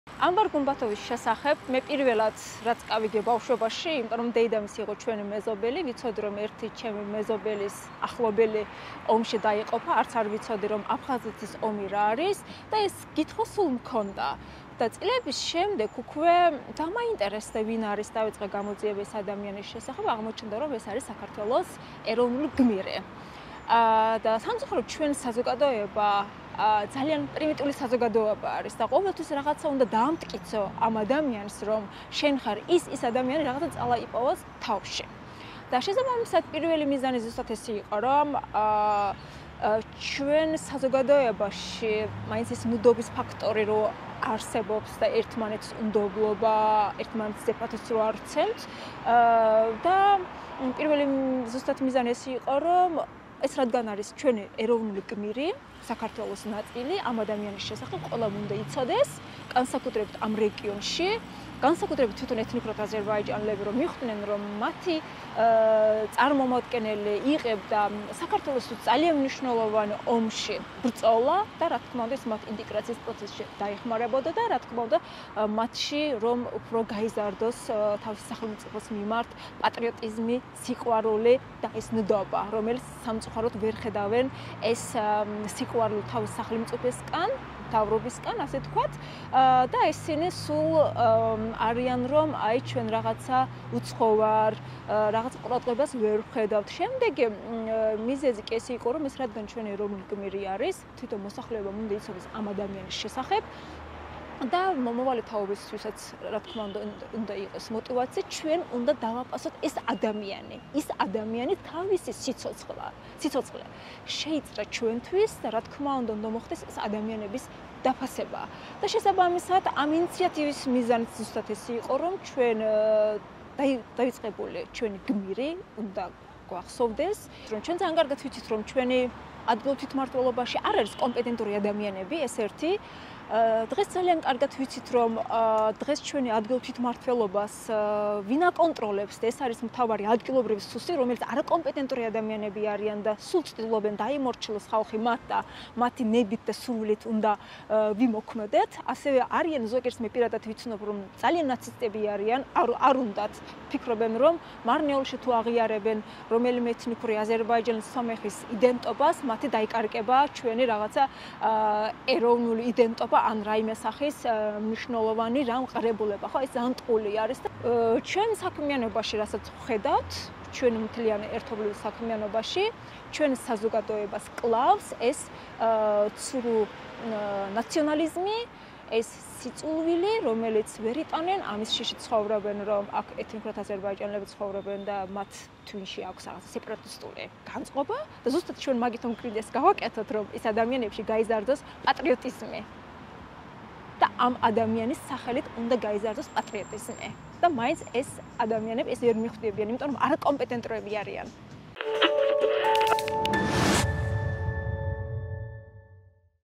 რატომ უნდა იყოს მარნეულსი იანვარ უმბატოვის ქუჩა/ძეგლი/მემორიალი
მარნეულელი უფლებადამცველი სამირა ბაირამოვა წლების განმავლობაში მუშაობს ეთნიკური უმცირესობების საკითხებზე. სამირა აცხადებს, რომ ადგილობრივი ეთნიკური უმცირესობის წარმომადგენლებში საქართველოსადმი კუთვნილების გრძნობისა და პატრიოტიზმის გასაღვივებლად საჭიროა ისნი ხედავდნენ თავინთ თავს საქართველოს ნაწილად.